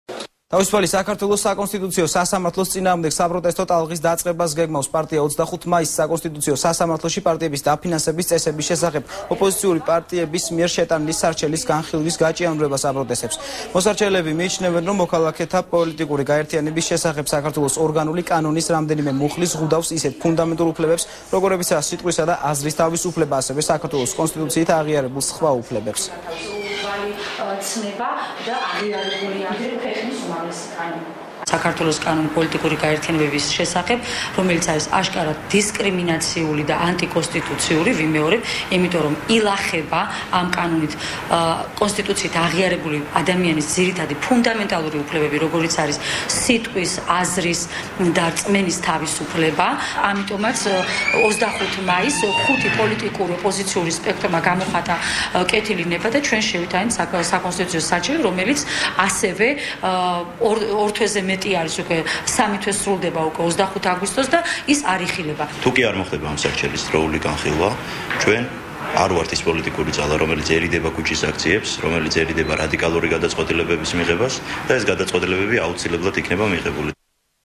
ახალი 6 | პროტესტი | 11.08.12
"თავისუფალი საქართველო" საკონსტიტუციო სასამართლოს წინააღმდეგ საპროტესტო ტალღის დაწყებას გეგმავს. პარტია 25 მაისს საკონსტიტუციო სასამართლოში პარტიების დაფინანსების წესების შესახებ ოპოზიციური პარტიების მიერ შეტანილი სარჩელის განხილვის გაჭიანურებას აპროტესტებს. მოსარჩელეები მიიჩნევენ, რომ "მოქალაქეთა პოლიტიკური გაერთიანების შესახებ" საქართველოს ორგანული კანონონის რამდენიმე მუხლი ზღუდავს ისეთ ფუნდამენტურ უფლებებს, როგორებიცაა სიტყვისა და აზრის თავისუფლება. ასევე საქართველოს კონსტიტუციით აღიარებულ სხვა უფლებებს.